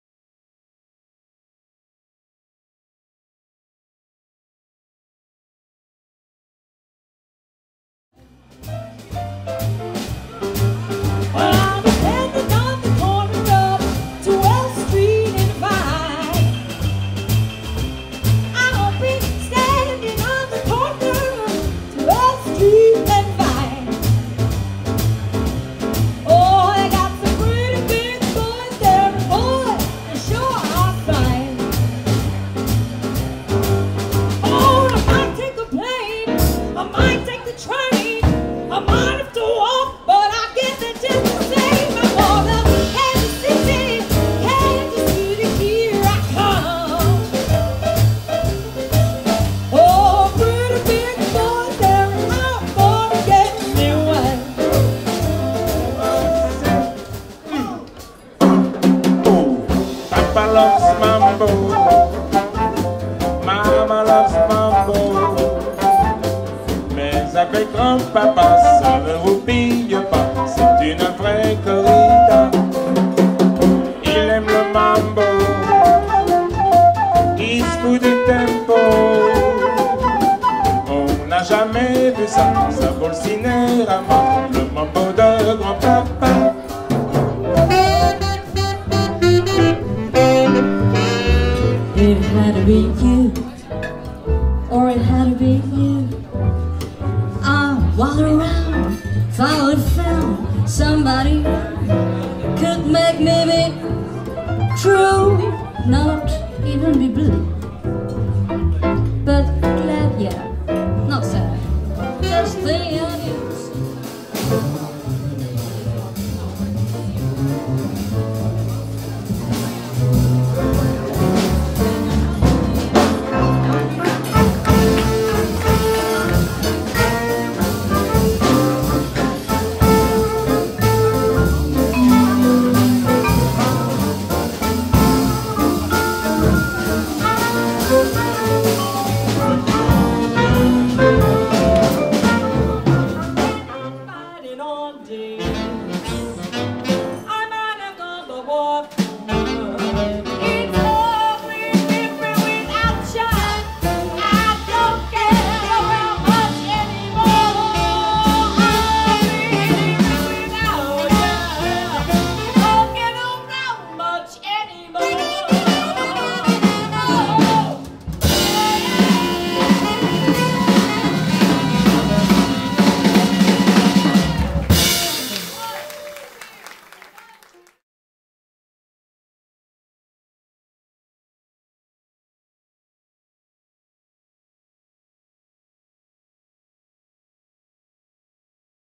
RIVERCAFE TV : JAZZ à la carte PARIS en Juin
Découvrez les Artistes présents lors des soirées Jazz du River Café  PARIS de JUIN 2013 : Nicolle Rochelle Swing Band, Ludovic De Pressac Swing Quintet, Christelle Pereira Quartet, Jazz For Ever, Gilda Solve .

Une vidéo de notre sonorisateur Florent Sorin.